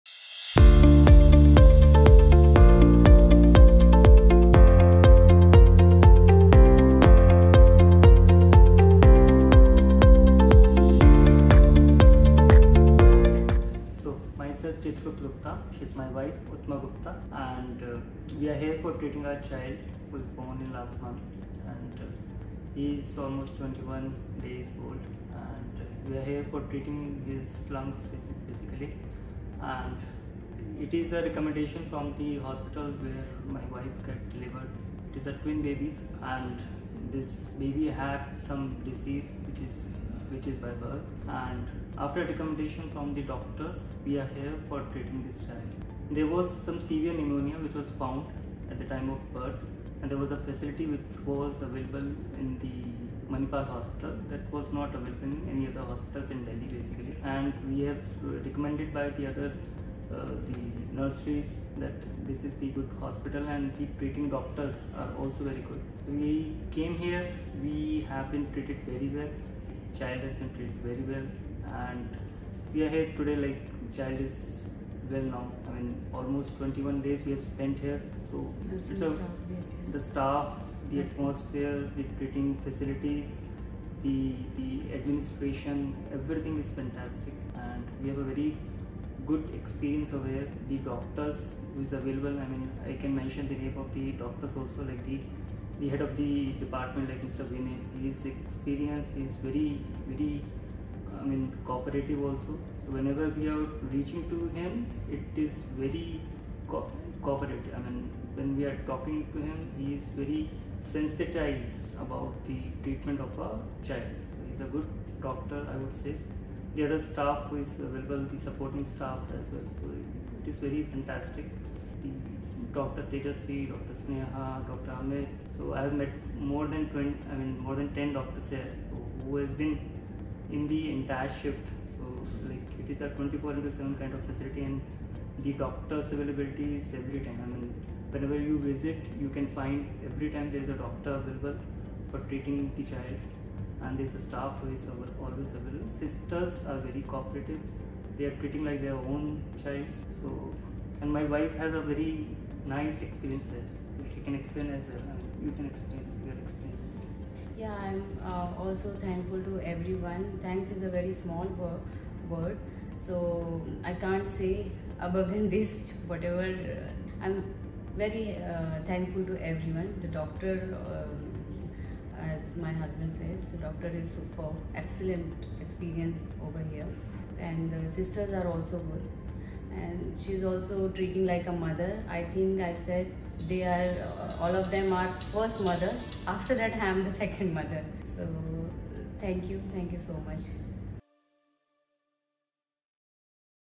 TPH Management | Nitric Oxide therapy | Dr Vinay Kumar Rai | Manipal Hospitals Delhi
Mr. Chirtup Gupta and his wife Mrs. Uttama Gupta decided to get their newborn baby treated at Manipal Hospitals Dwarka for severe pneumonia.

Watch the video as the couple thanks Dr. Vinay Kumar Rai, Consultant - Neonatology, Pediatrics, and the medical team at the hospital for their dedicated and gentle care that made the baby recover soon.